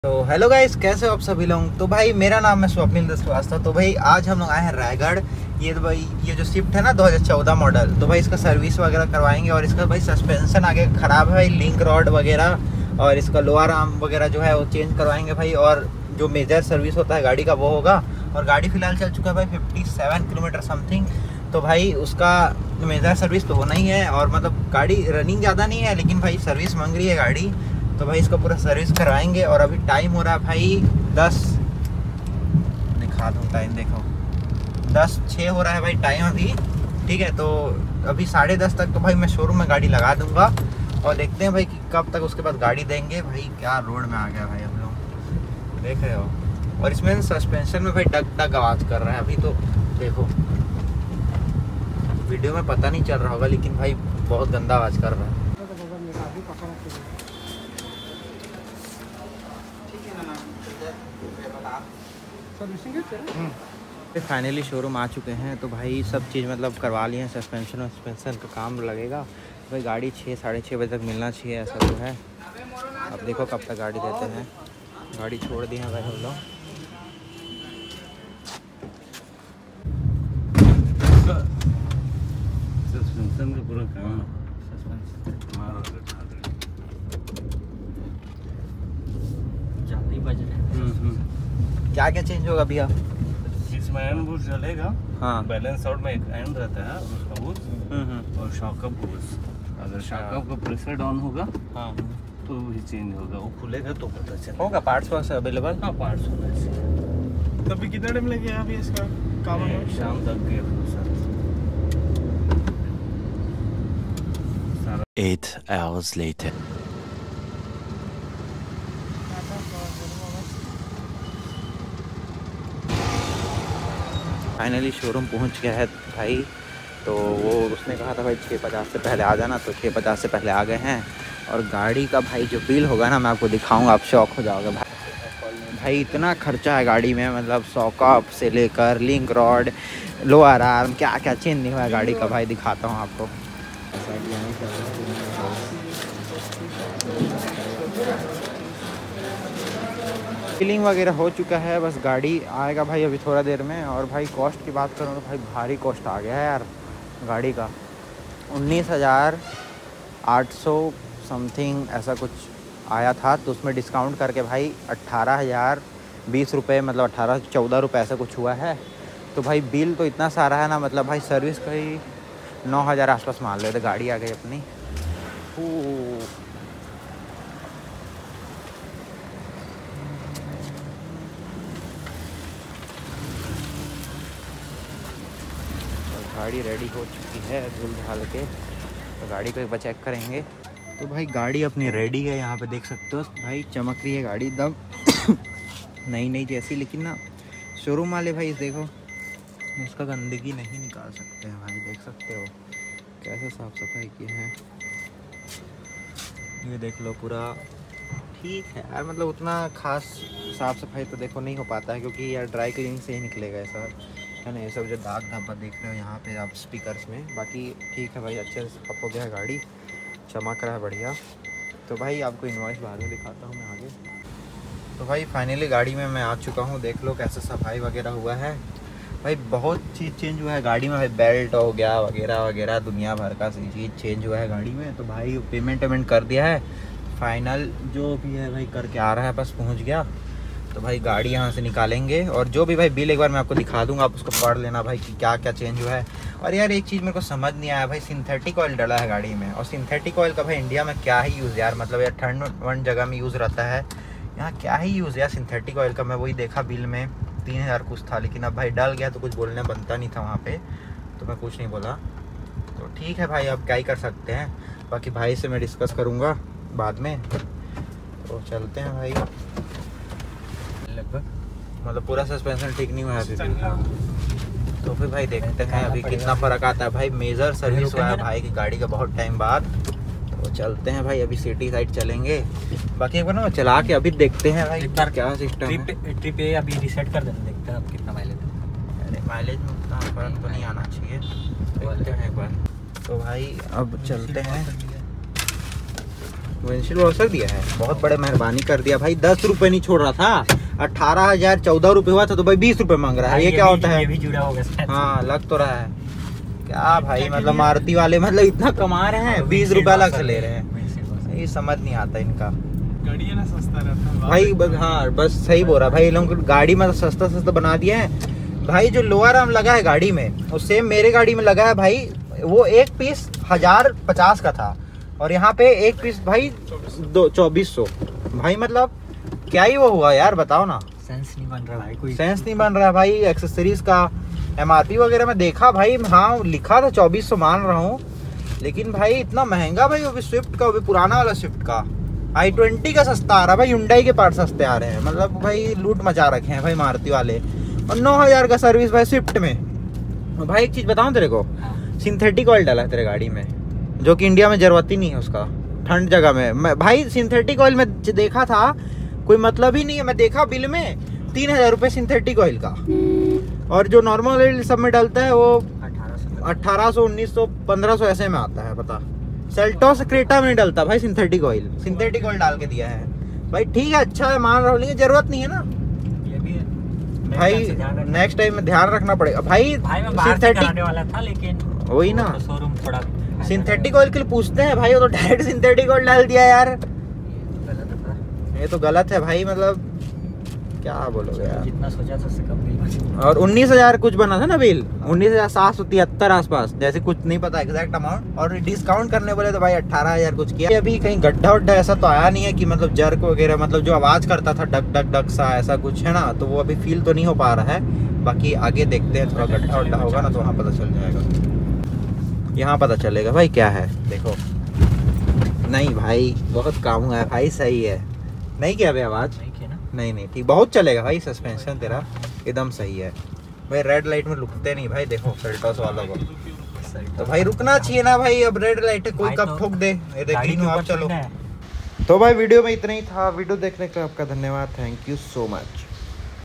Paid service done Swift 57000 kms || suspension work done for swift.
Hello guys
To bhaii iss video me maii apko swift 2015 model ka major paid service dhikane wala hu jisme bhaii …
Suspensions bhi change hua hai bhaii to video me pura cost wagera bataunga apko

       - Swapnil Dev Shrivastava.